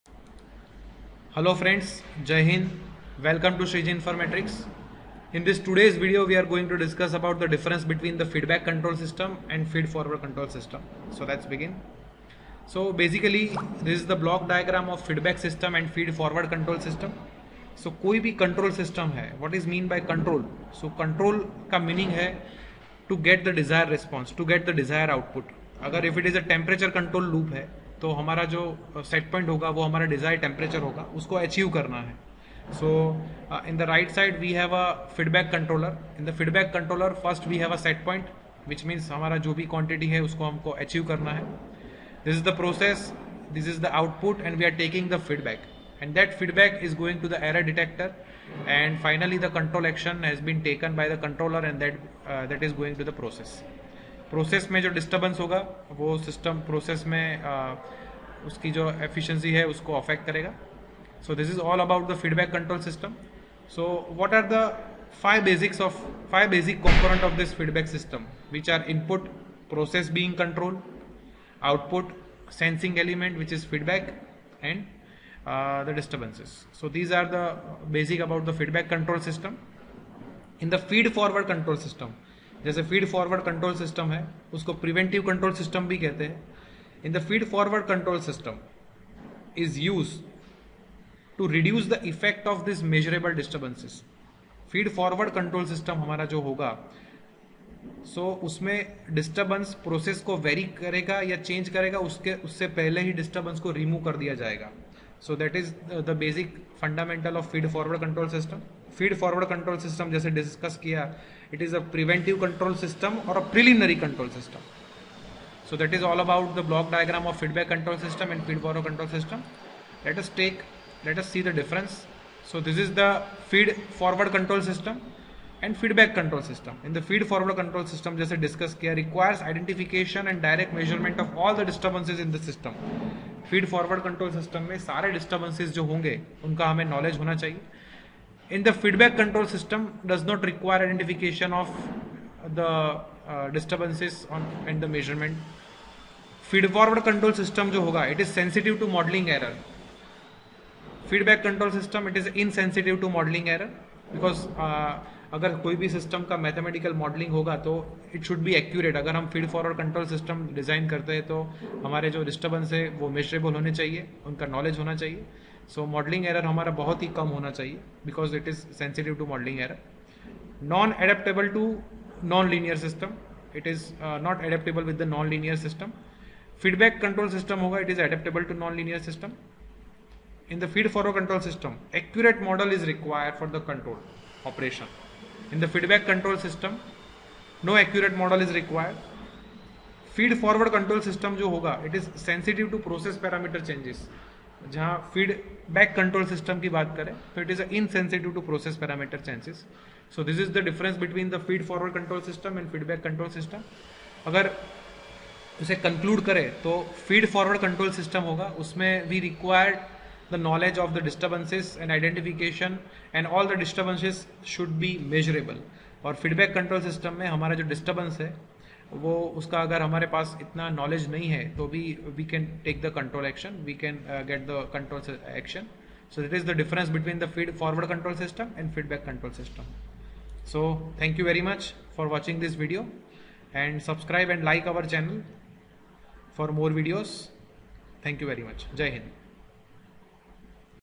Feedback Control System Vs. feedforward Control System||Difference between feedback and feedforward
A control system has two main task: get the system to track a set-point, and eliminate disturbances. Feedback control is  powerful for this, but this video shows how feed-forward control can make achieving those goals easier.